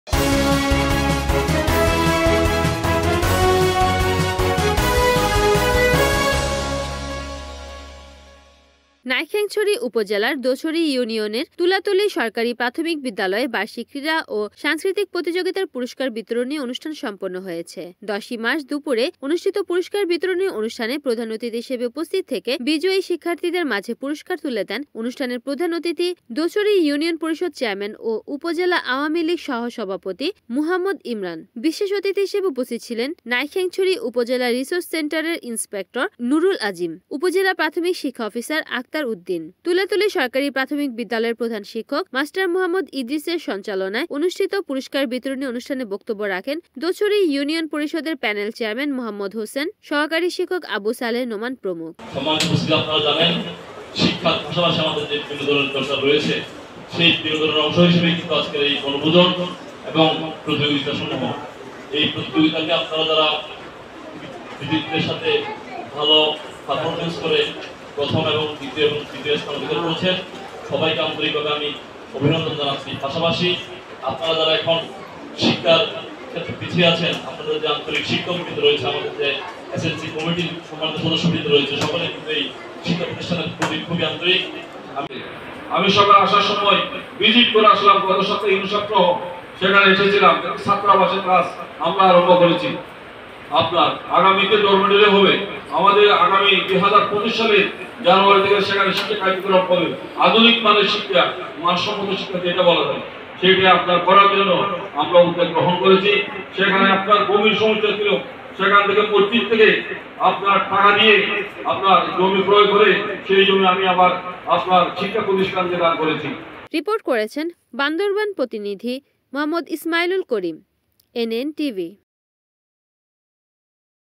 তুলাতলি সরকারি প্রাথমিক বিদ্যালয়ে বার্ষিক ক্রীড়া ও সাংস্কৃতিক প্রতিযোগিতা পুরস্কার বিতরনী || NAN TV
Official Address:
76/2 Uttor Chashara, Rajdhani Apurbo Tower,